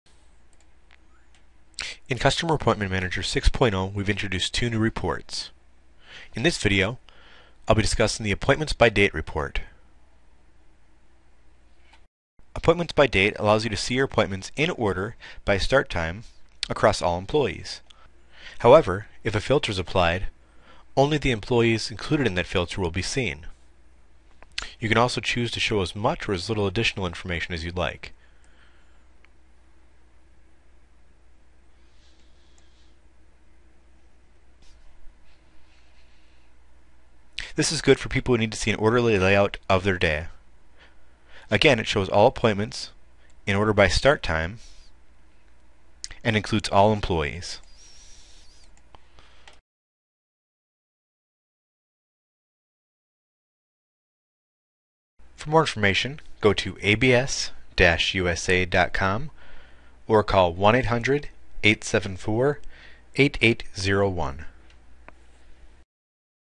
Appointments By Date Report in Customer Appointment Manager Version 6.0
The Appointments By Date report is new in Customer Appointment Manager version 6.0. Learn more about our appointment software

appointment scheduling software